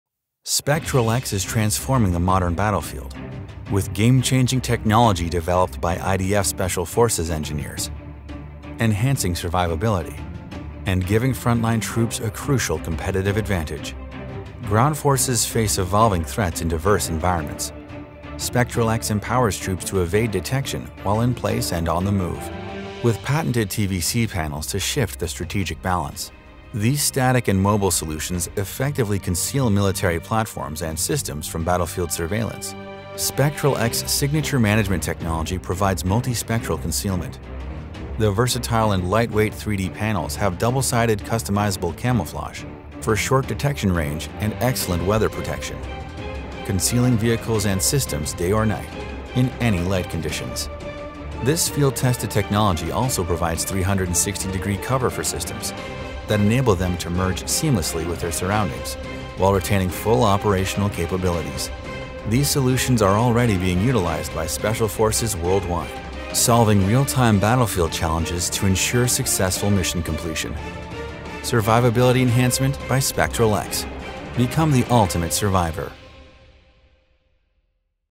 TITAN Branding. Branding SpectralX
Branding & website design SpectralX. SpectralX Land Video.
Mission Critical Entire Spectrum Survivability.
When the mission is everything, Spectralx innovates the technologies that make the impossible possible.